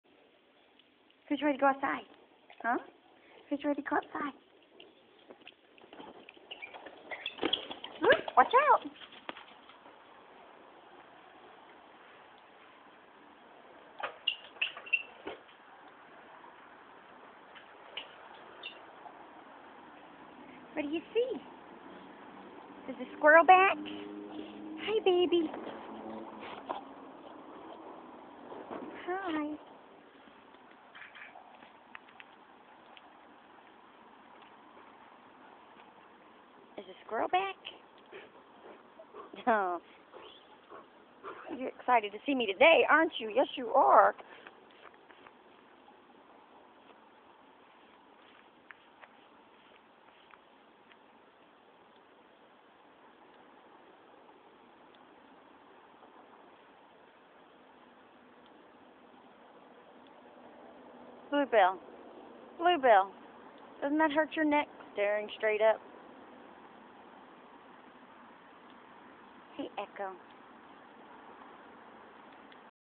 Echo and Blue Bell Quick Visit
Two Newfoundlands enjoying there visit